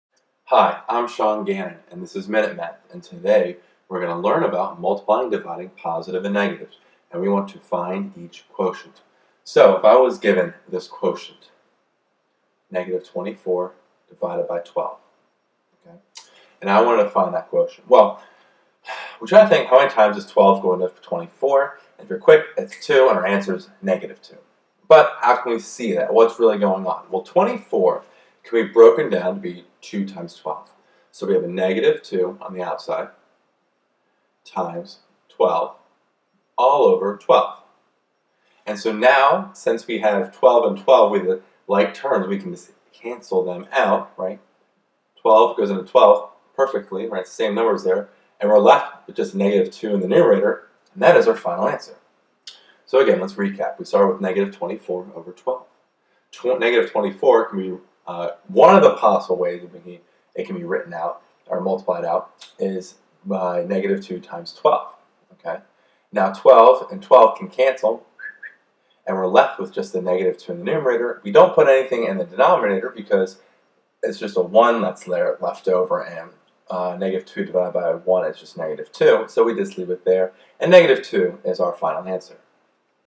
Divide -24/12
In this math video lesson on Multiplying and Dividing Positives and Negatives, I find the quotient of -24/12.